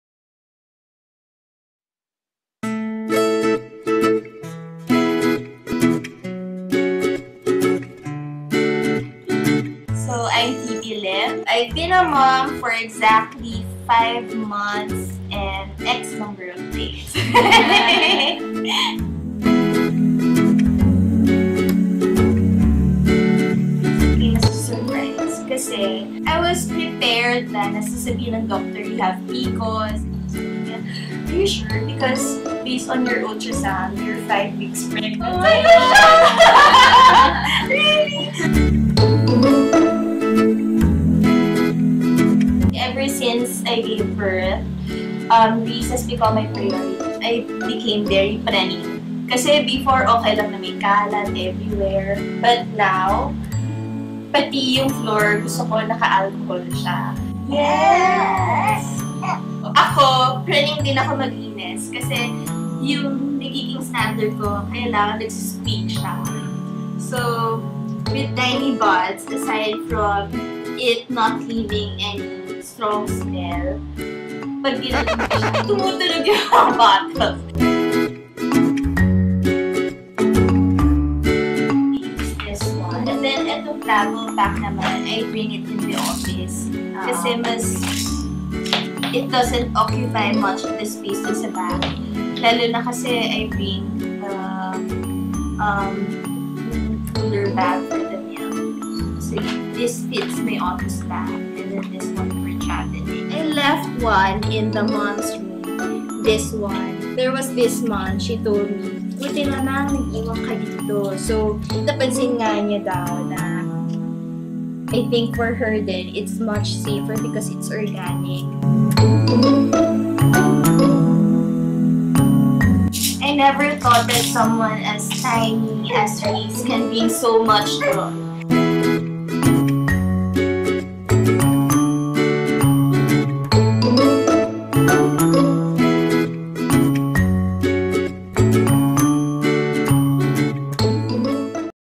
Phoebe's Tiny Bud | Stories from a Working Mom
Phoebe Lim is the perfect example of Work Life Balance

She is a model employee at one of the largest & most fast paced telecom companies in the country.

She recently gave birth for the first time to a beautiful baby girl named Reese.

She is a perfectionist in mounting big presentations & rushing deadlines so it comes as no surprise that even for play time and feeding time, the tinyest things matter.

TINY BUDS BABY NATURALS
Natural to the Tinyest Drop!